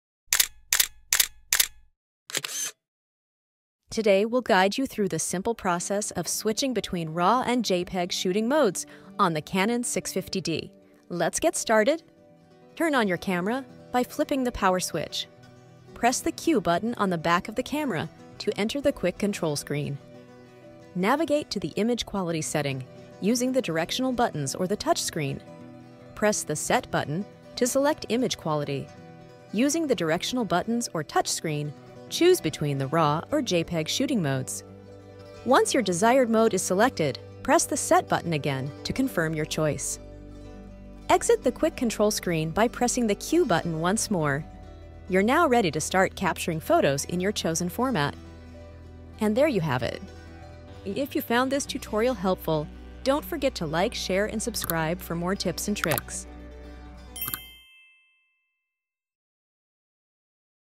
Mastering Image Quality: Raw vs. JPEG Image Formats with Canon EOS 650D!
Confused about whether to shoot in Raw or JPEG on your Canon EOS 650D? This video breaks down the differences between Raw and JPEG image formats, helping you make an informed decision for your photography needs. Learn the advantages and disadvantages of each format and discover which one suits your shooting style and post-processing workflow best. Maximize the capabilities of your Canon EOS 650D and elevate your photography game with this essential guide!